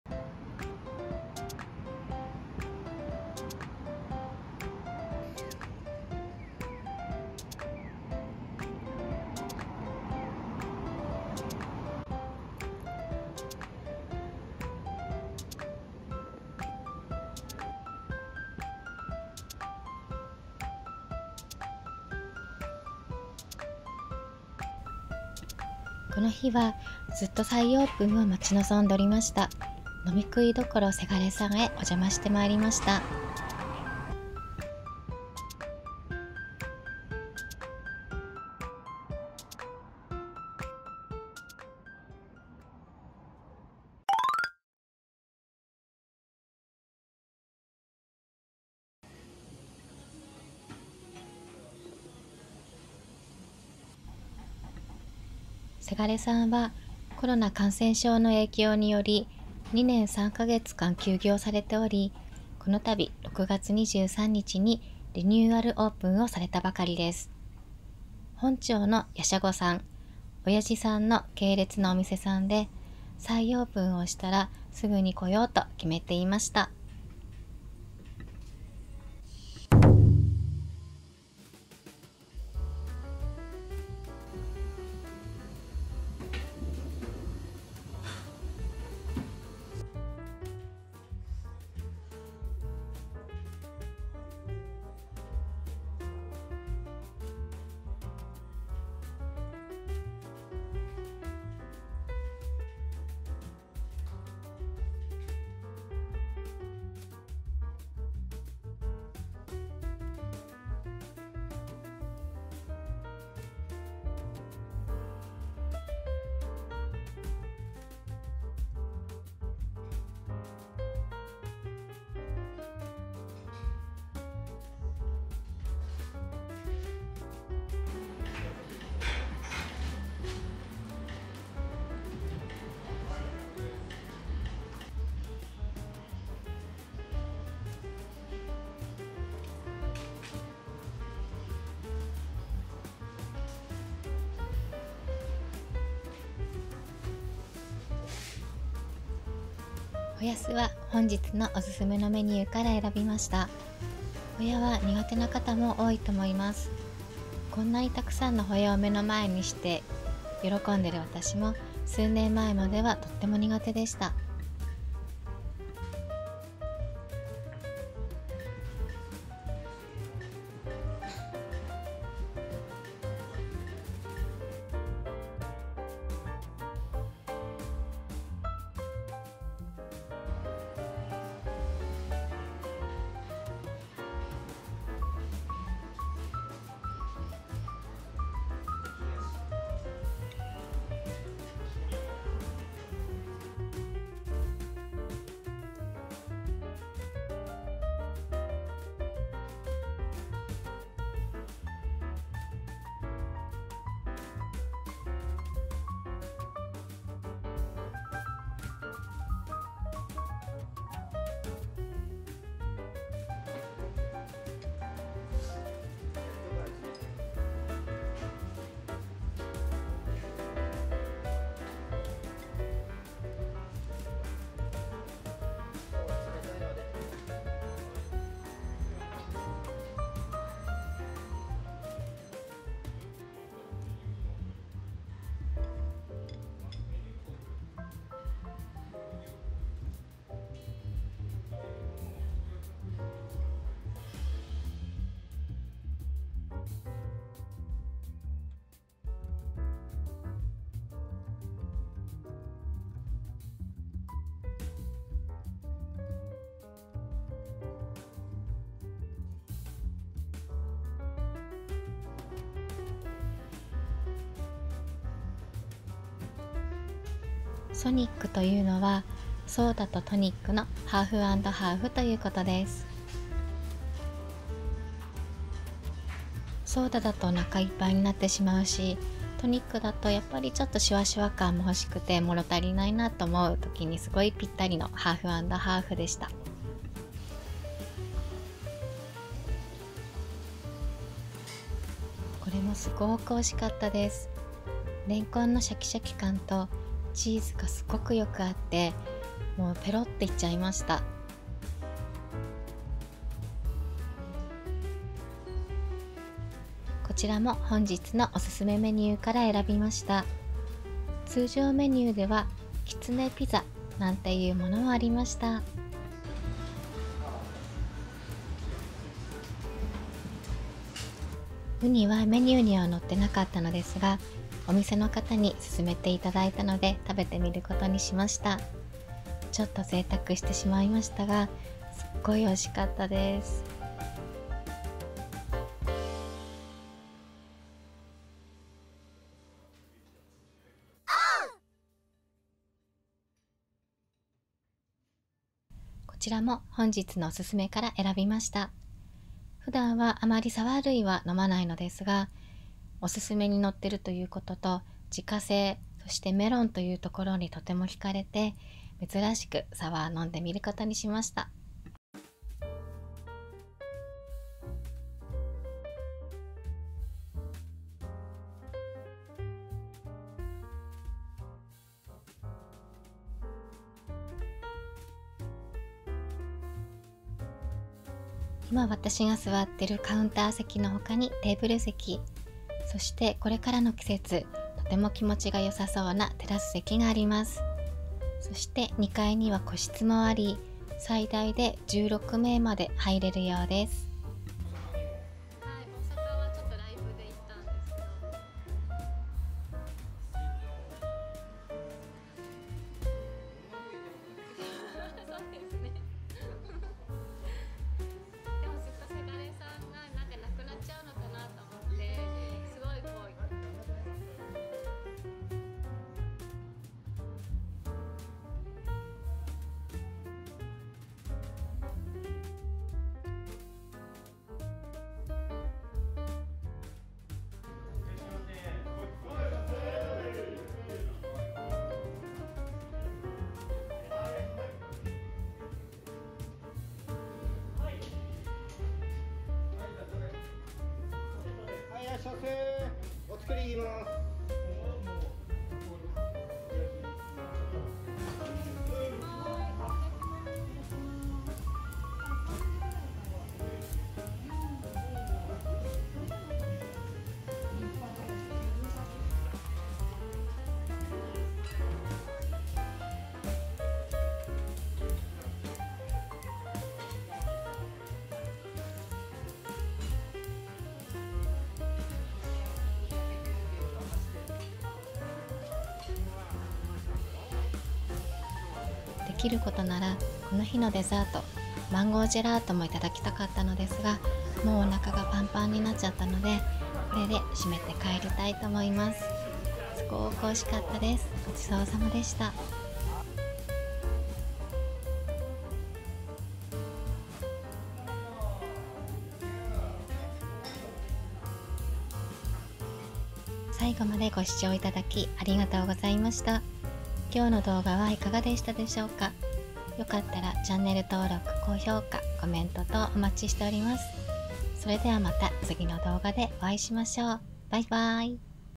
【待望のリニューアルオープン！】2年3ヶ月ぶり！素敵な一軒家居酒屋さんでちょっと贅沢な一人飲み【がぜうに】
🍺2年3ヶ月ぶりにリニューアルオープンした、待望の一軒家居酒屋さんで一人飲みしてきました。

🍀住所や営業時間等記載してみました。変更なども考えられますのでその点はご了承ください♬

＊3密にならないよう感染症対策をしっかりして行動しています。
＊外食を促進してはおりません。動画をご覧いただいて、行ったような気持ちになっていただけたり、家族ご飯や状況収束後の参考にしていただけたら嬉しいです。